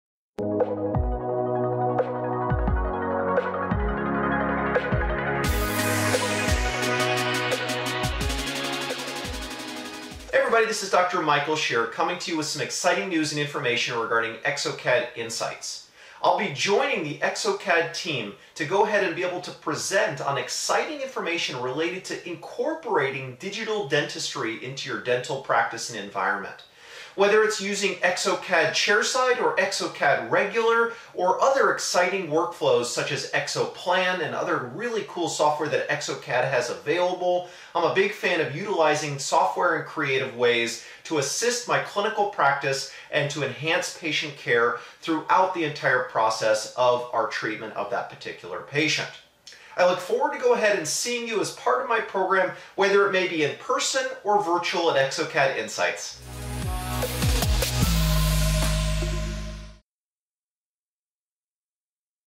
Meet Dr. Michael Scherer @ exocad Insights 2020!
Don't miss Dr. Michael Scherer's lecture @ exocad Insights 2020!

Topic: What a Dentist Can Do With exocad
Date: September 21, 2020
Time: 17:25 - 18:10 (CET)

In his guest lecture, Dr. Scherer will discuss the role of exocad in his combined clinical and laboratory dental practice and how the software can be used in daily clinical practice.
Interested in learning more? Secure your ticket for exocad Insights via Eventbrite

for further information.